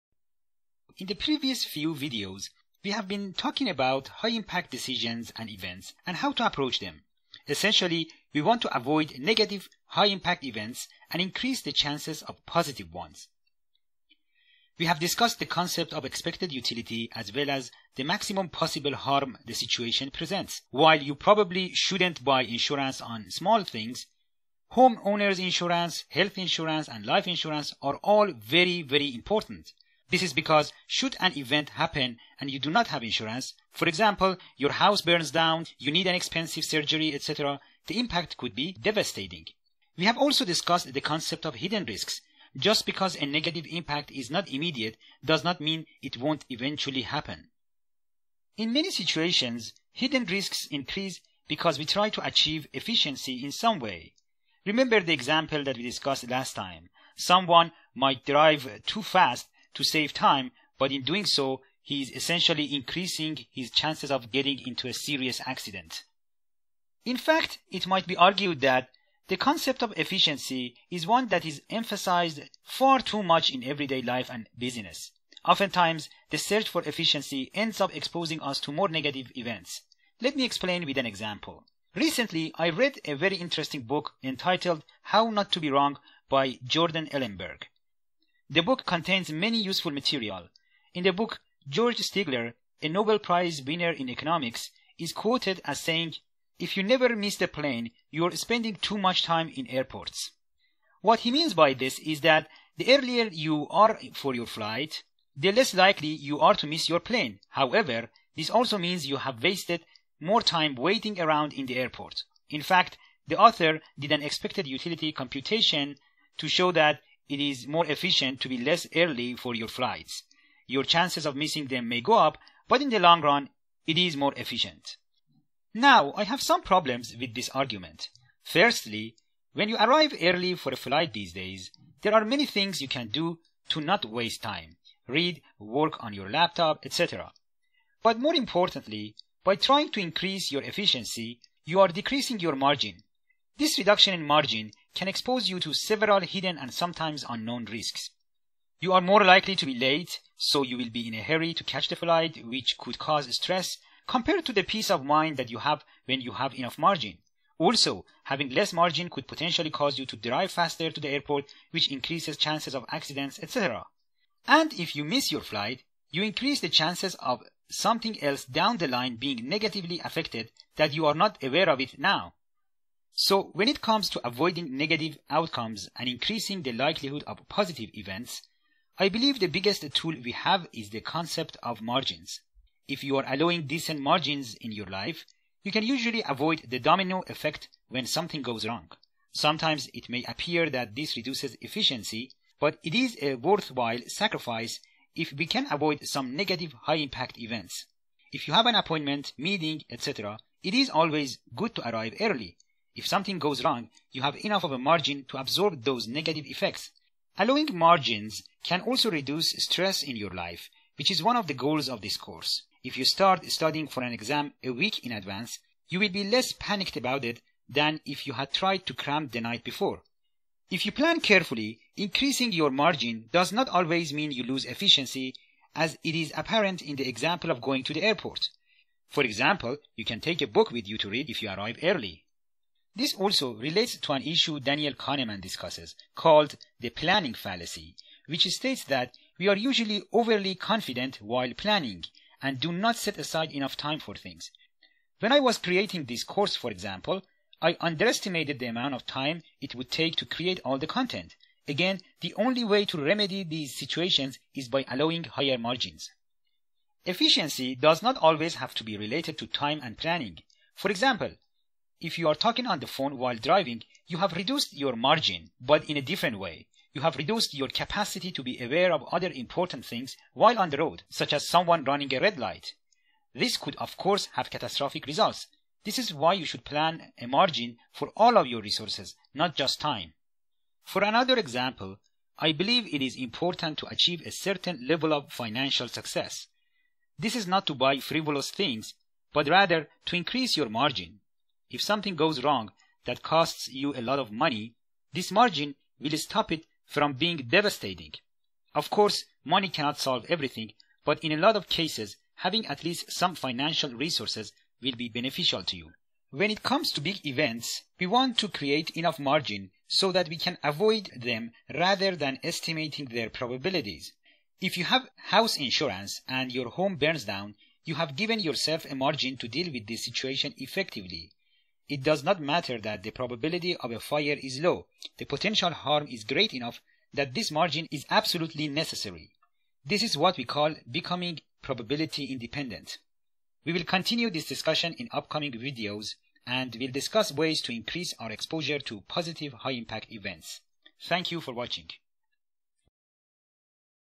Lecture 37: Efficiency and Its Flaws
This video discusses efficiency and the negative events that can result from focusing too heavily on it. Using an example of arriving late to an airport to save time, too great a focus on efficiency can lead to a series of issues. The concept of margins is discussed as a method to combat the negative aspects of efficiency. The planning fallacy is also discussed.

Further reading: “Antifragile”, by Nassim Taleb and “Thinking, Fast and Slow” by Daniel Kahneman.

Section 10: High-Impact Decisions and Events (3/4)

This is the 37th of a total of 40 short videos, which are divided into 11 sections.

A course to learn the basic concepts and tools to help us make better decisions under uncertainty, take calculated risks, and reduce the stress and regrets that often come with decision making.